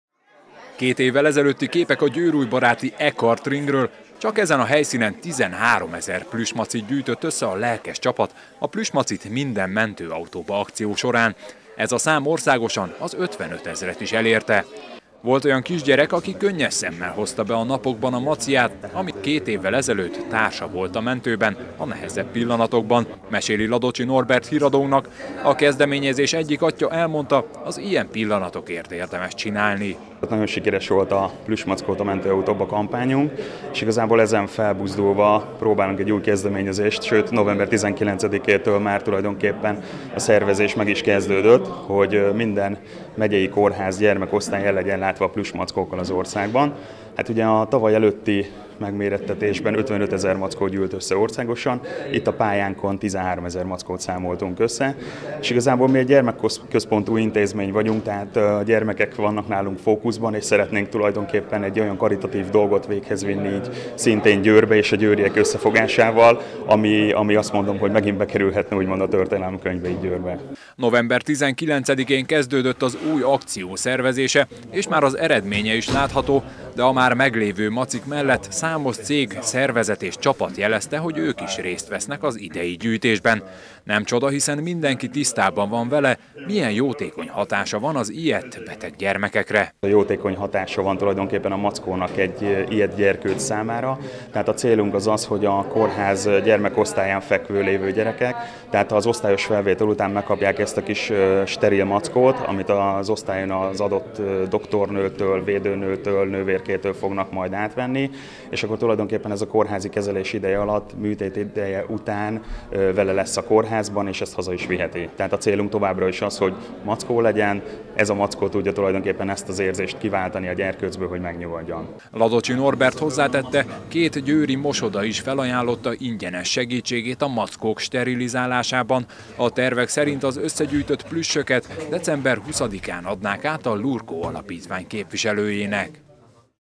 Plüssmacikat a kórházakba is!
Újra plüssmacikat gyűjt a győrújbaráti E-kart Ring csapata. Két évvel ezelőtt országos szinten több mint 55 ezret sikerült összegyűjteniük, a „Plüss macit minden mentőautóba” akció során, most a kórházak gyermekosztályainak toboroznának, még ennél is többet. A macik ugyanis oldják a beteg vagy balesetet szenvedett gyermekek feszültségét, egyfajta társként számíthatnak rá a legkisebbek.